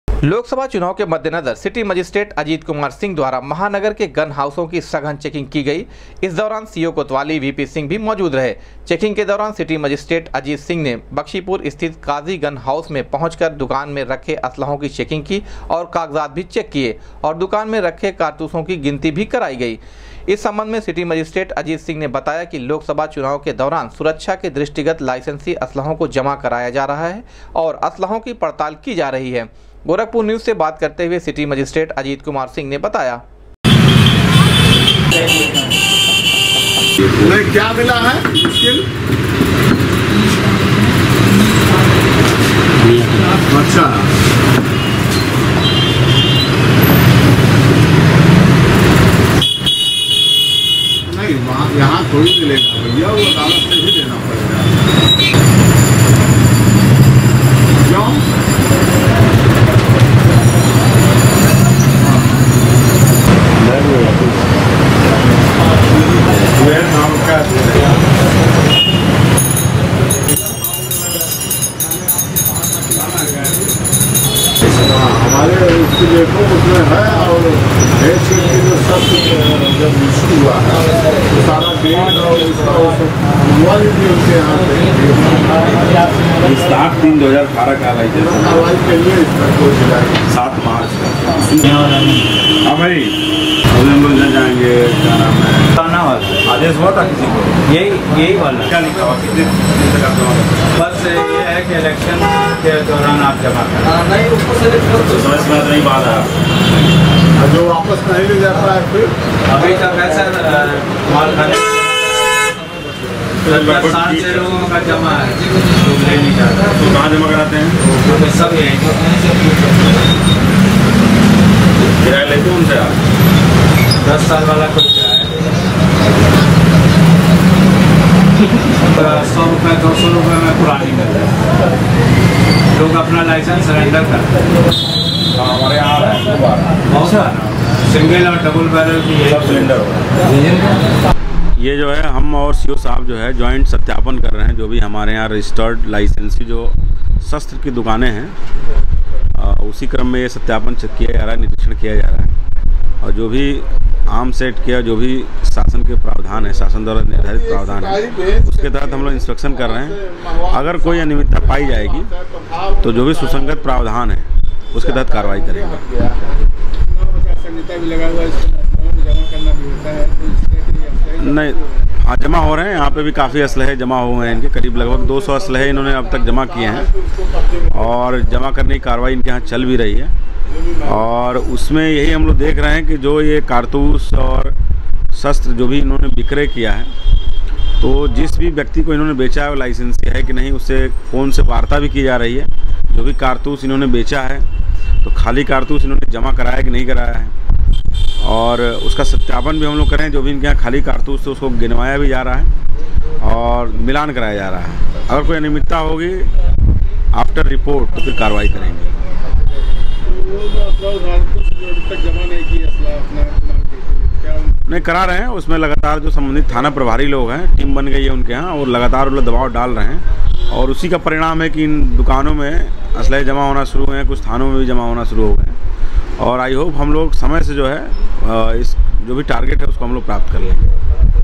लोकसभा चुनाव के मद्देनजर सिटी मजिस्ट्रेट ने महानगर के गन हाउसों की की सघन चेकिंग
लोकसभा चुनाव के मद्देनजर सिटी मजिस्ट्रेट ने महानगर के गन हाउसों की की सघन चेकिंग, कहा सुरक्षा की दृष्टिगत की जा रही है चेकिंग